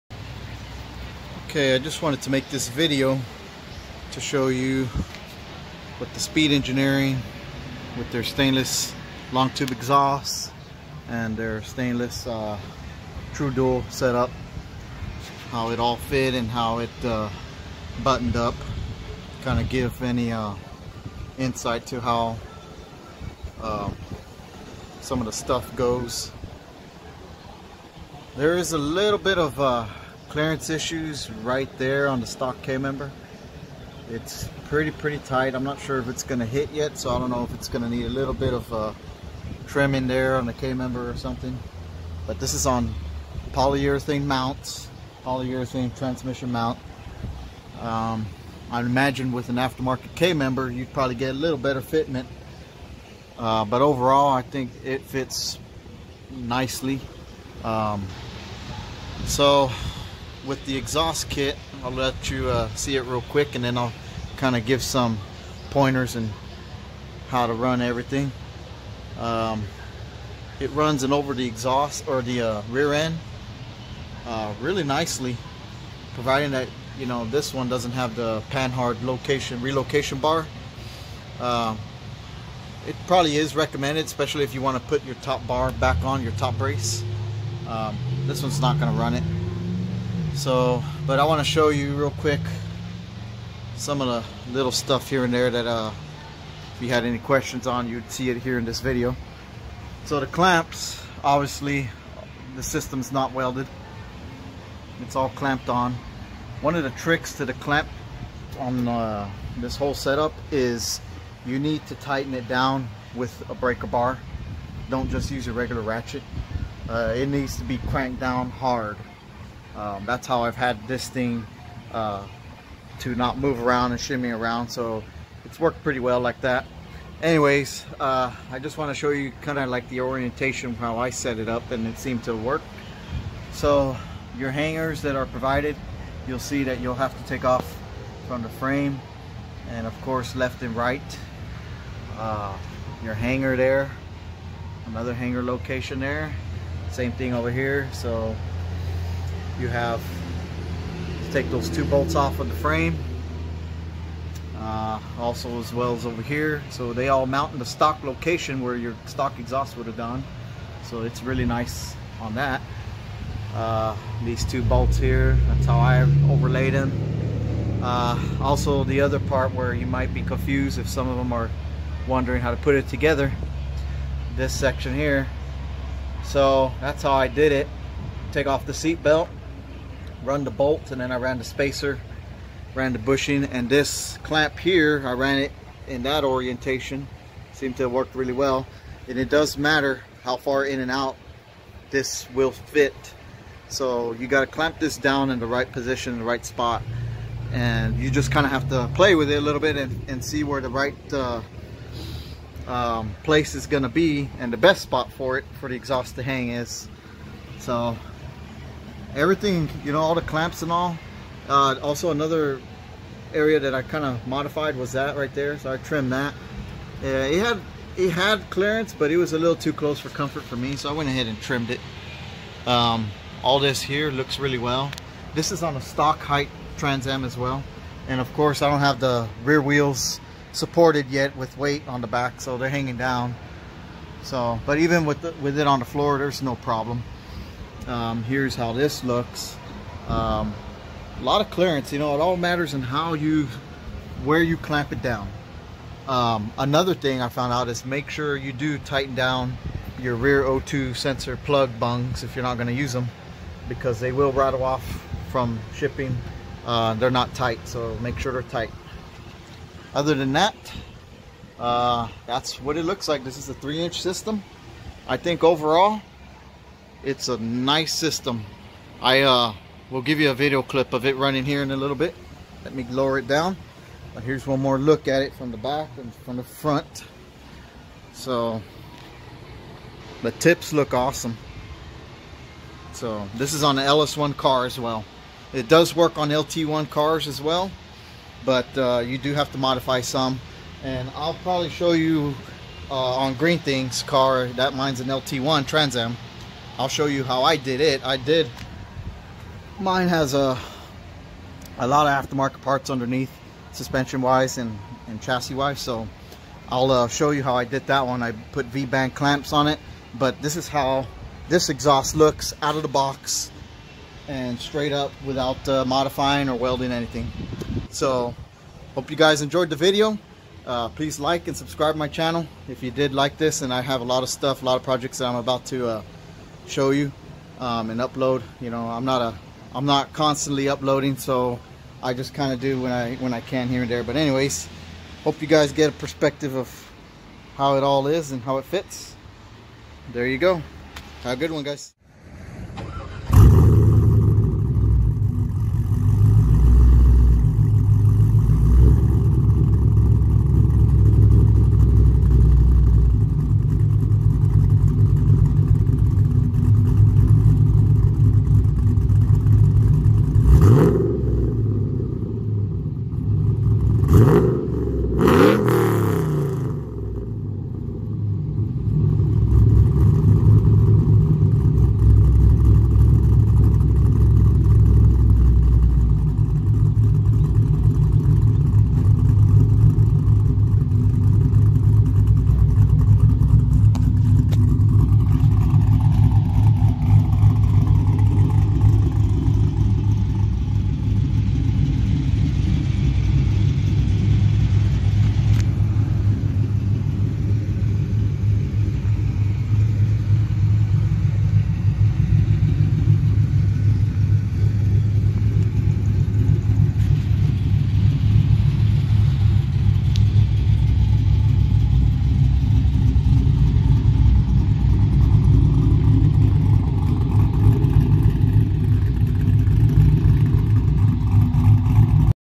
Speed engineering true dual stainless exhaust setup on LS1
Speed engineering true dual exhaust stainless steel with their 1 7/8 stainless long tubes  headers on a LS1 underside and rev and walk around